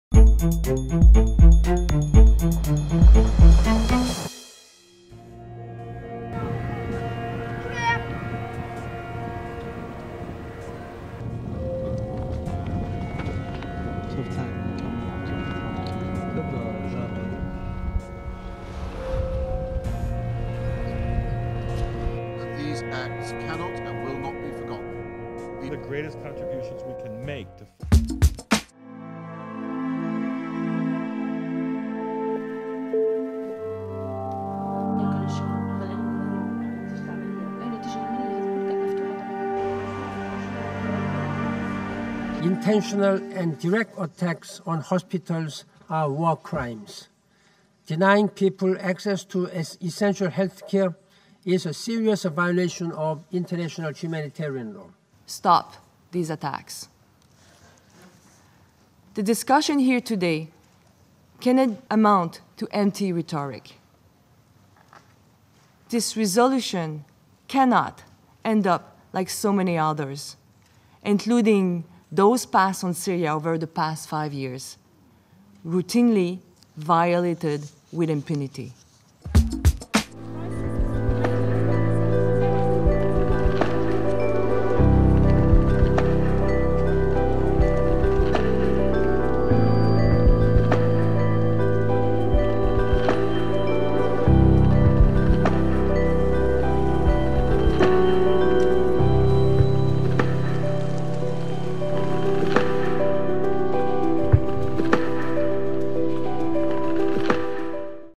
Resumo semanal da ONU em imagens #87
O conflito na Síria continua provocando inúmeras mortes e deslocamentos em massa o Conselho de Segurança da ONU aprova resolução para proteger profissionais de saúde, pacientes e hospitais em zonas de guerra a caça ilegal no Quênia matou 100 mil elefantes entre 2010 e 2012 – estes são os destaques do resumo semanal da ONU em imagens. Confira neste vídeo.